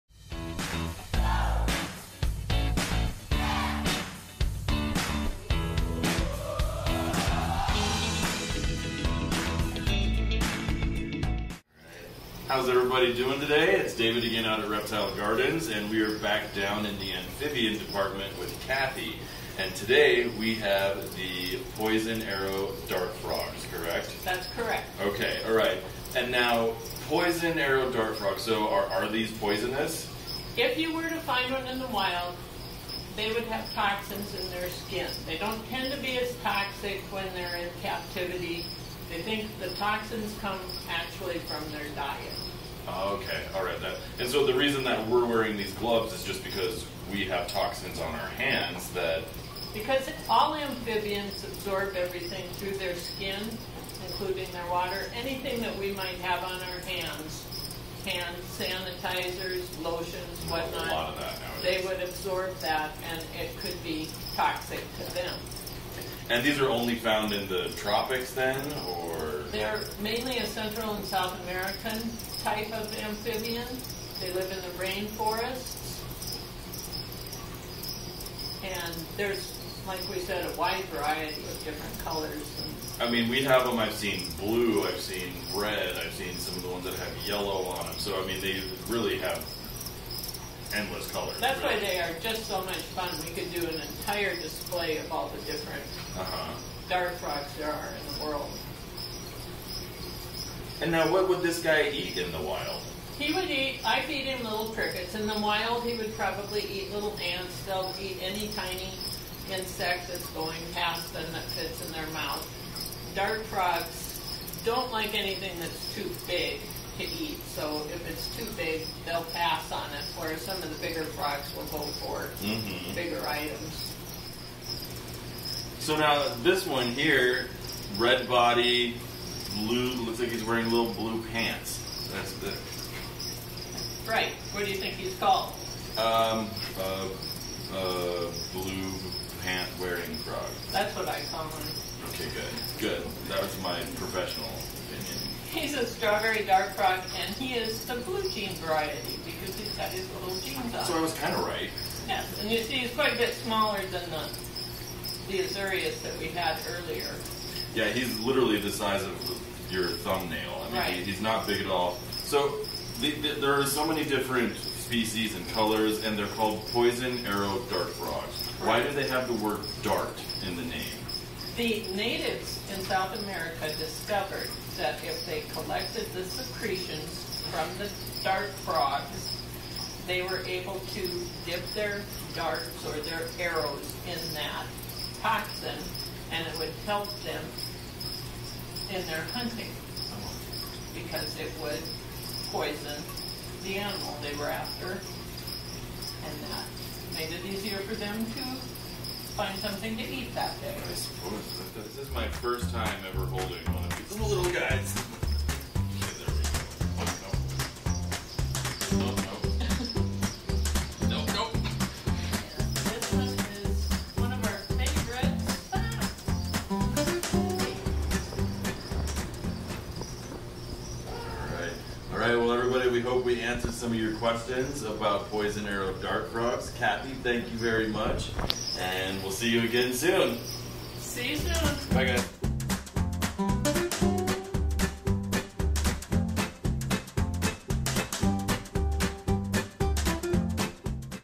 David Learns Stuff: Poison Dart Frogs
Explore with head curator of horticulture, David, as he learns about the animals we have at Reptile Gardens. Today's topic: poison dart frogs.